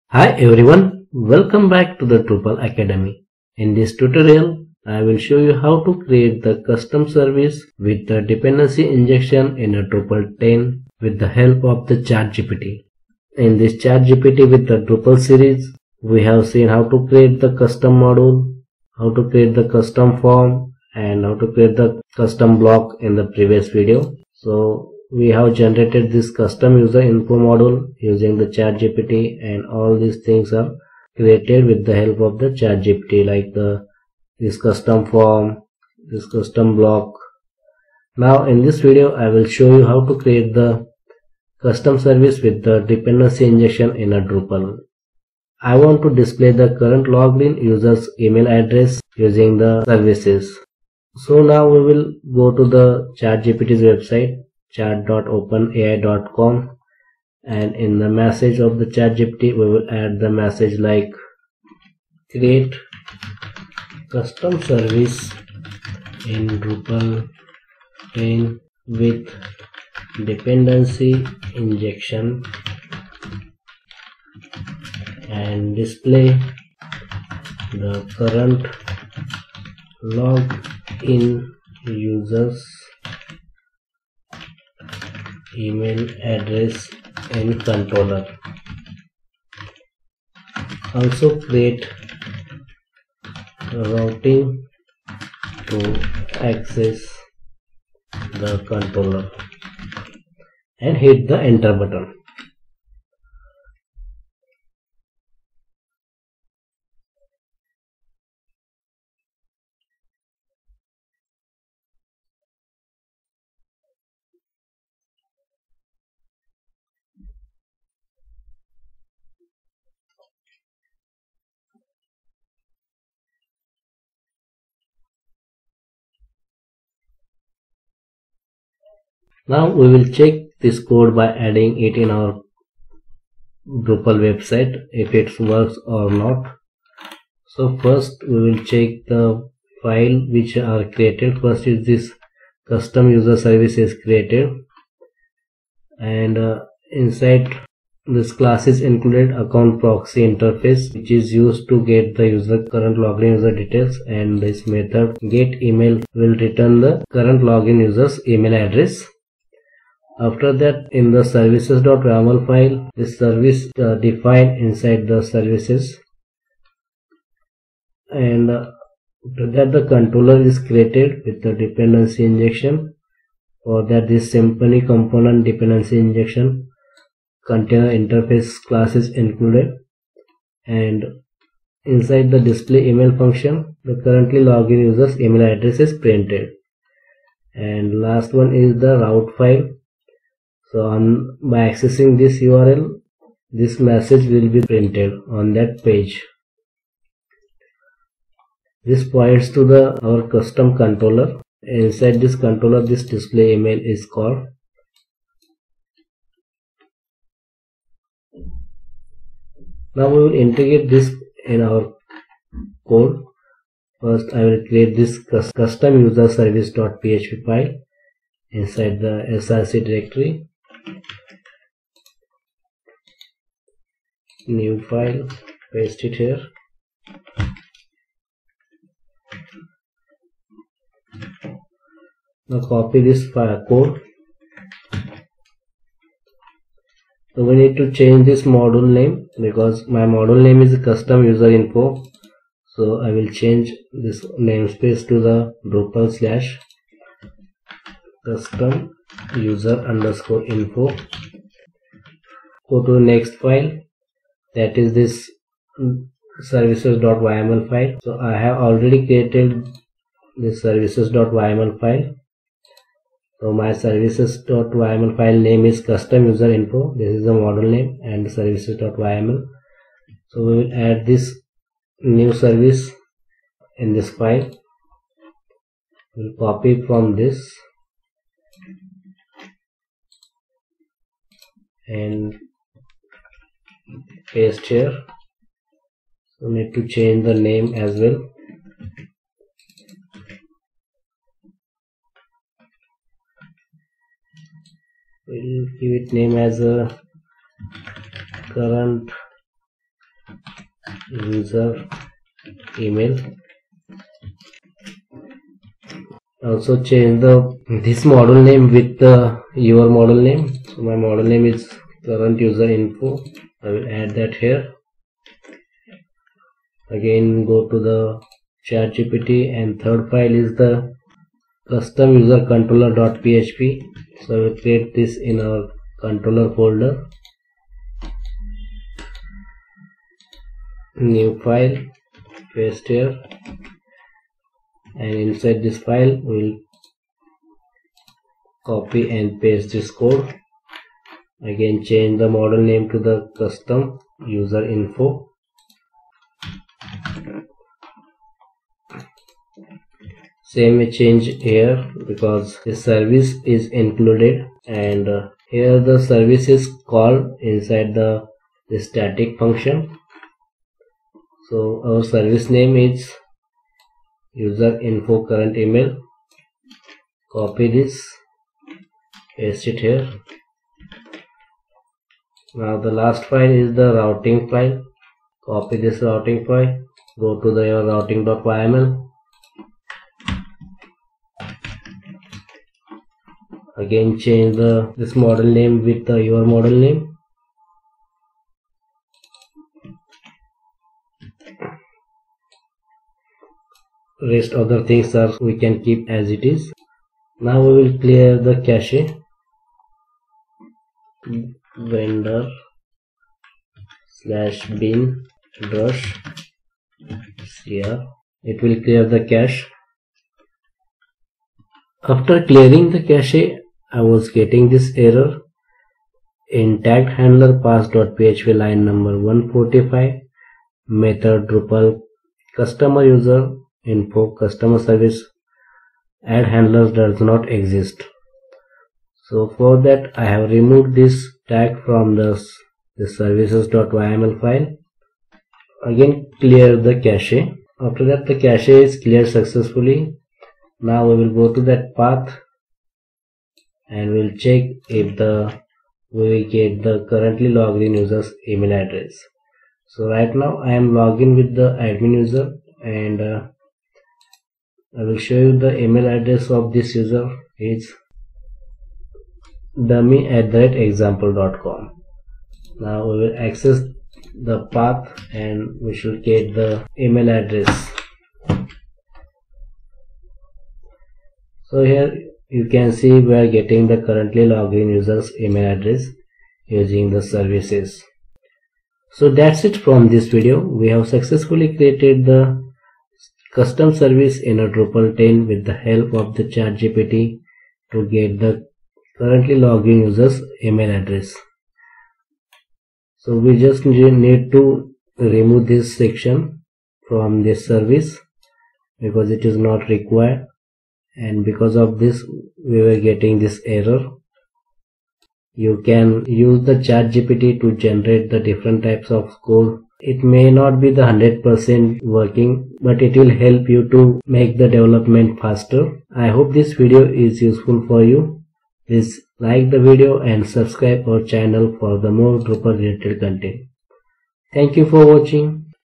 Create custom service with dependency injection in Drupal 10 using ChatGPT
Dependency injection in Drupal.

In this tutorial, we'll walk through creating a custom service in Drupal 10 using dependency injection.

🔵 Steps Covered:

Creating a new module: custom_user_service.
Registering the CustomUserService as a service.
Defining a route to access the controller.
Enabling the module and accessing the route.
By the end of this tutorial, you'll have a better understanding of how to create and utilize custom services in Drupal 10, and how to display user-specific data in a controller.

▬▬▬▬▬▬ Other Drupal Videos 📹  ▬▬▬▬▬▬